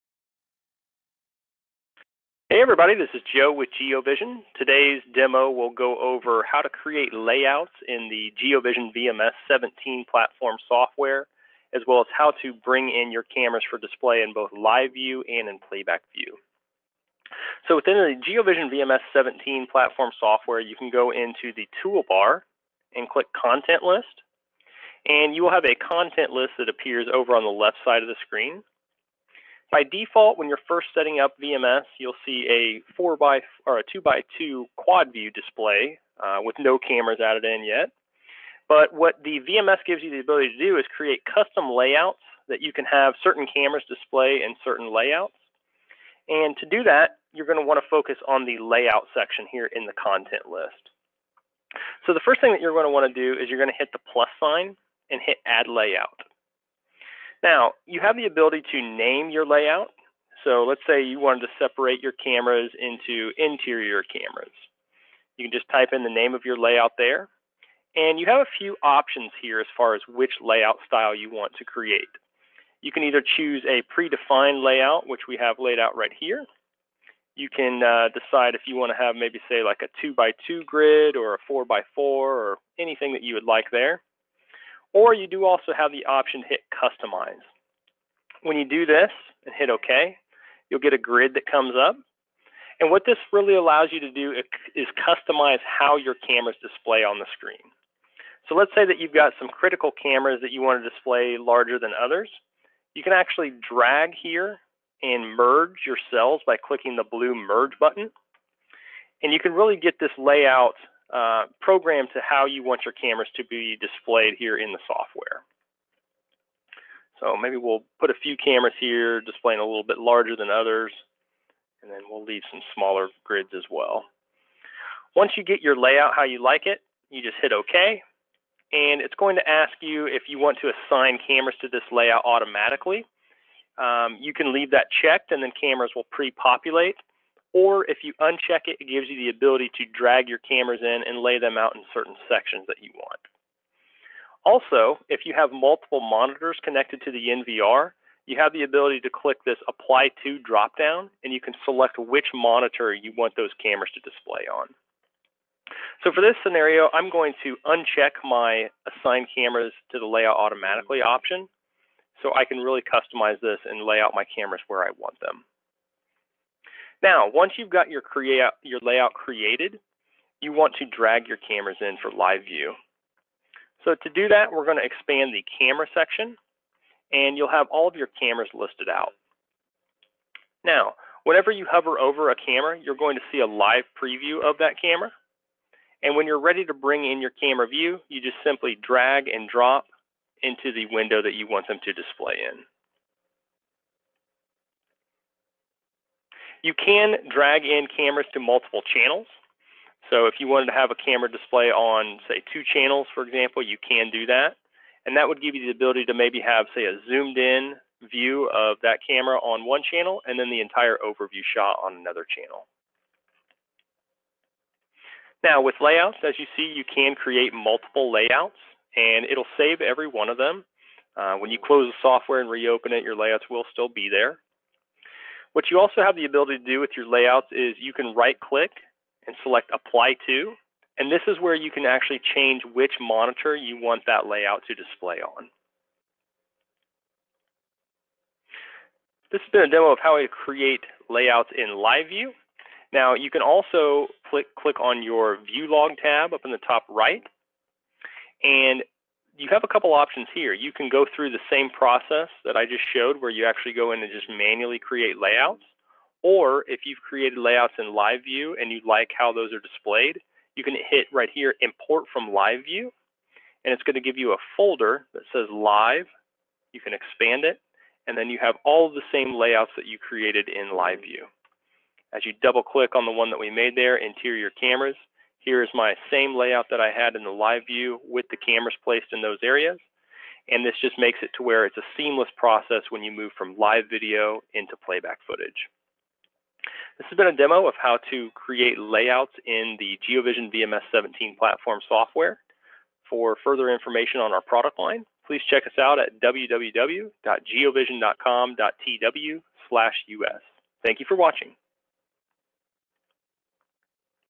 VMS - Creating Layouts
This is a video tutorial of how to create custom camera layouts in the GeoVision VMS software. Multiple layouts can be saved in the live and playback window allowing users to be able to quickly access sets of cameras upon the click of a mouse.